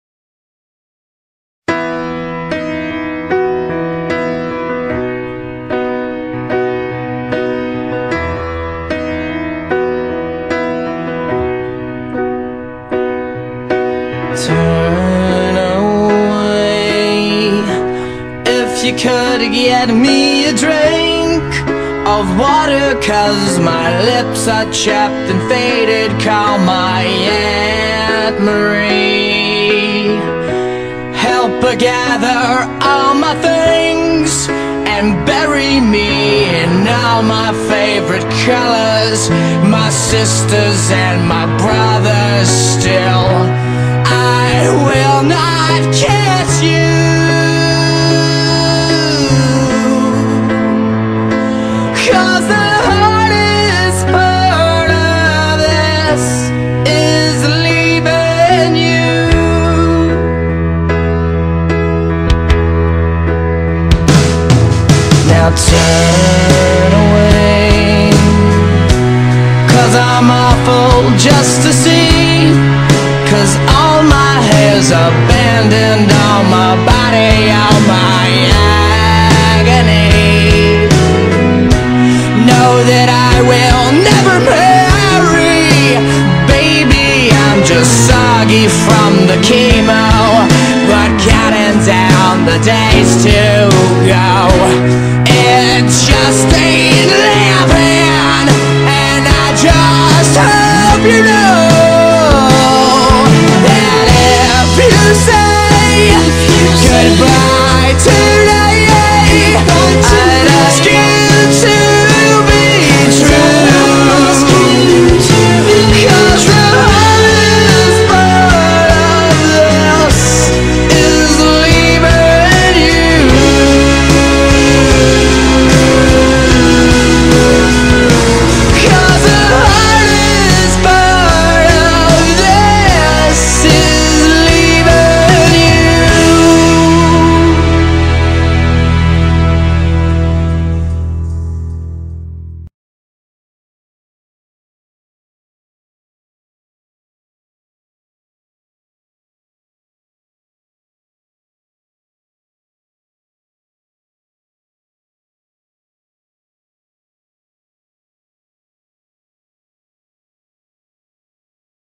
My Chemical Romance Tribute
(music is owned by WMG. no copy right infringments intended)
This goes out to all my friends. Hayden, Joe, Ryan, Kira, Katie and Ben. i know you guys probubly dont like this song... but i really wanted to make this one. so i hope you like it to.

(again. THIS IS NOT MY MUSIC!!)